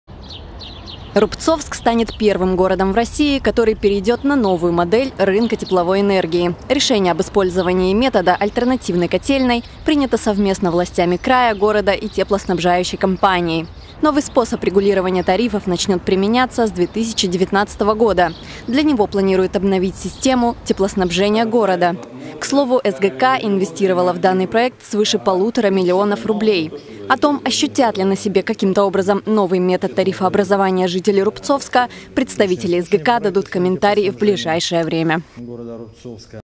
Рубцовск переходит на новую модель рынка тепловой энергии
Соответствующее решение об использовании метода «альтернативной котельной» было принято совместно властями края, города и СГК. Новый способ регулирования тарифов начнет применяться с 2019 года.